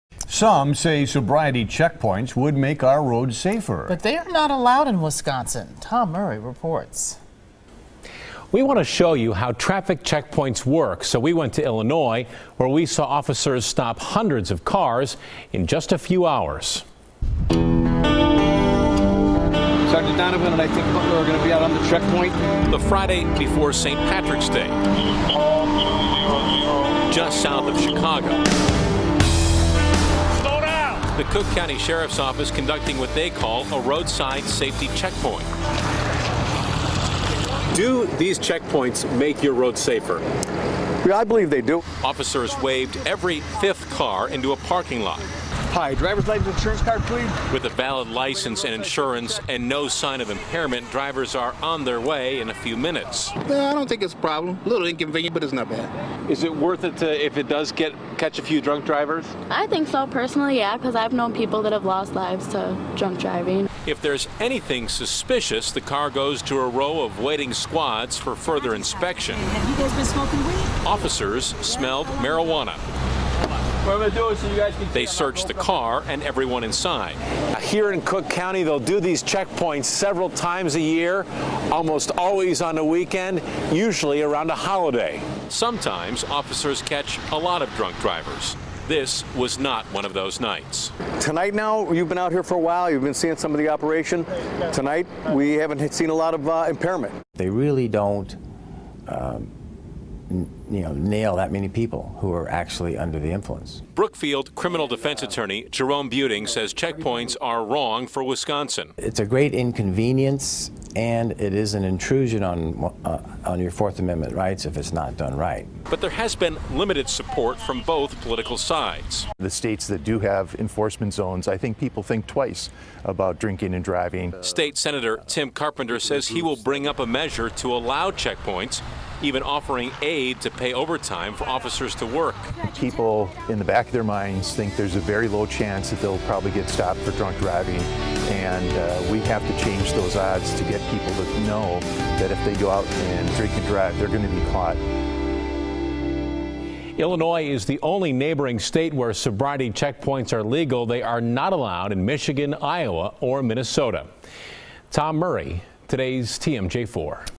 Sobriety checkpoints: How they work
We want to show you how traffic checkpoints work. So we went to Illinois--where we saw officers stop hundreds of cars in just a few hours.